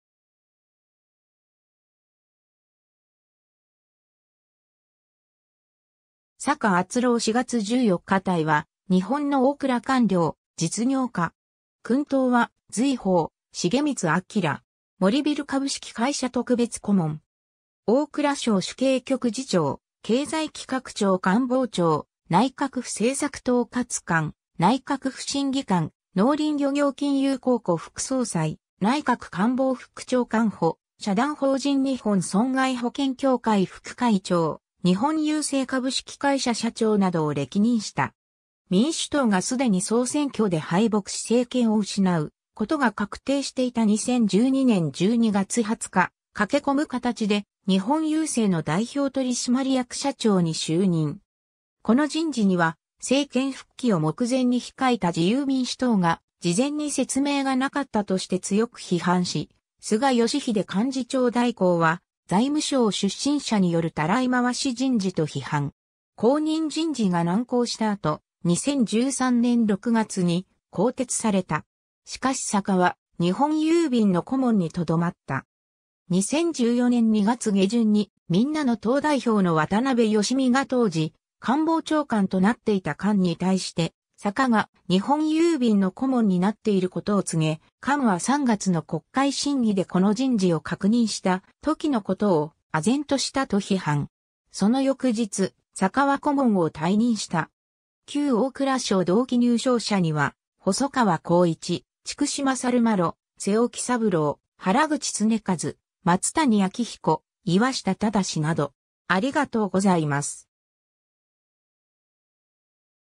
坂篤郎
坂 篤郎（さか あつお、1947年（昭和22年）4月14日 - ）は、日本の大蔵官僚、実業家。
勲等は瑞宝重光章。
森ビル株式会社特別顧問。
 大蔵省主計局次長、経済企画庁官房長、内閣府政策統括官（経済財政・経済社会システム担当）、内閣府審議官、農林漁業金融公庫副総裁、内閣官房副長官補、社団法人日本損害保険協会副会長、日本郵政株式会社社長などを歴任した。
 民主党が既に総選挙で敗北し政権を失うことが確定していた2012年12月20日、駆け込む形で日本郵政の代表取締役社長に就任。
この人事には政権復帰を目前に控えた自由民主党が事前に説明がなかったとして強く批判し、菅義偉幹事長代行は財務省出身者によるたらい回し人事と批判。
後任人事が難航した後、2013年6月に更迭された。
しかし坂は日本郵便の顧問にとどまった。
 2014年2月下旬にみんなの党代表の渡辺喜美が当時官房長官となっていた菅に対して、坂が日本郵便の顧問になっていることを告げ、菅は3月の国会審議でこの人事を確認した時のことを「唖然とした」と批判。
その翌日、坂は顧問を退任した。
 旧大蔵省同期入省者には、細川興一（元財務事務次官）、筑紫勝麿（元財務省造幣局長）、妹尾喜三郎（元ビックカメラ取締役副会長）、原口恒和（イオン銀行会長）、松谷明彦（政策研究大学院大学名誉教授）、岩下正（元国際協力銀行理事）など。